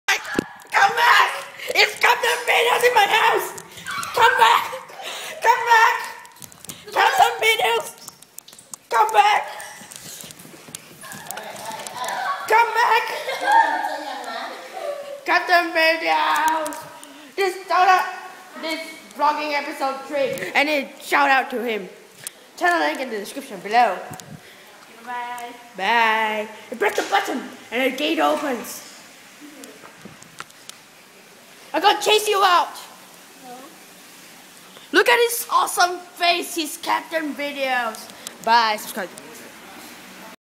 Ep3:Shout out to captainvideos2002
He came over to play minecraft

Go checkout his channel. He is AWSOME!! Go sub to him